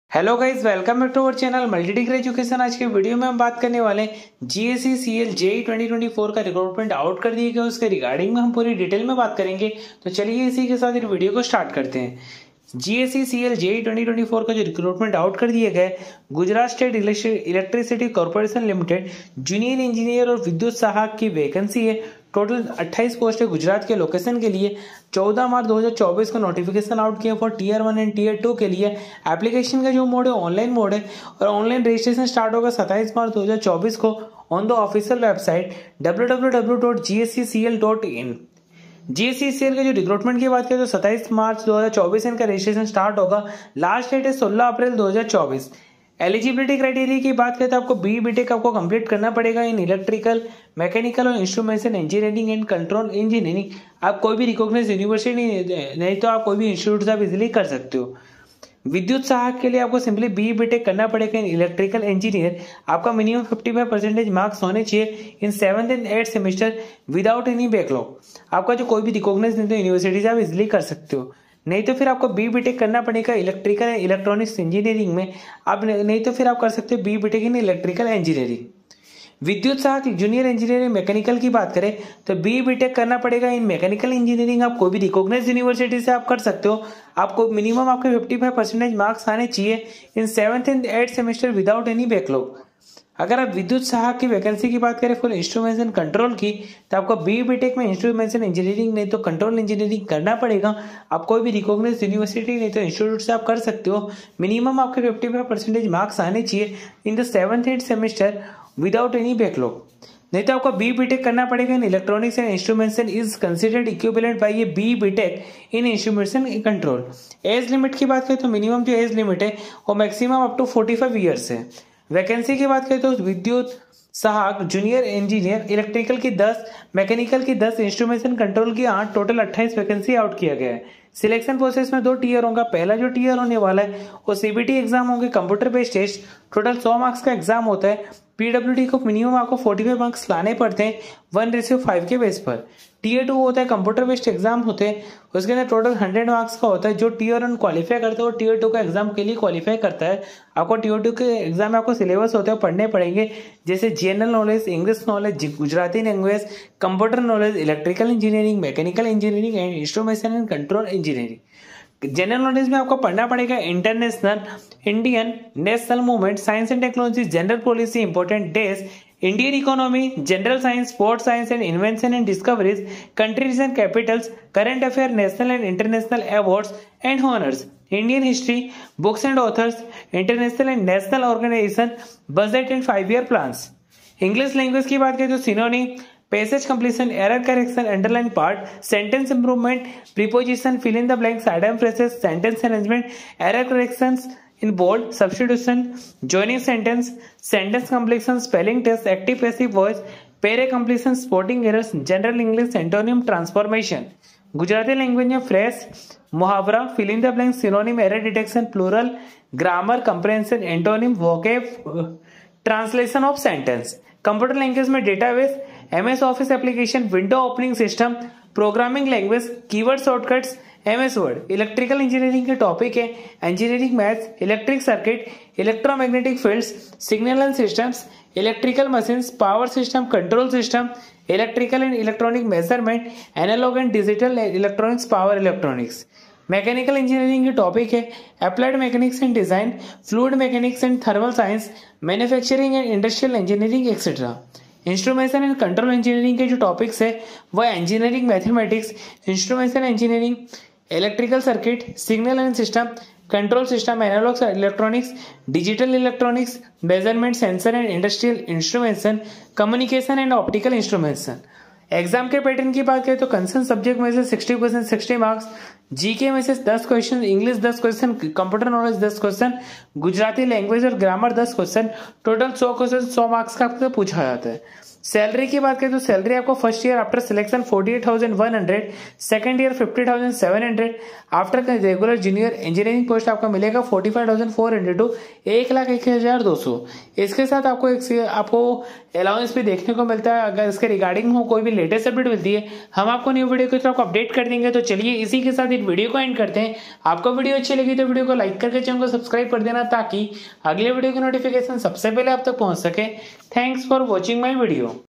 GSECL Recruitment 2024 | Notification Out | Full Detail Explained | MDE
GSECL 2024 Official Notification Out Full Details Explaination by multi degree education.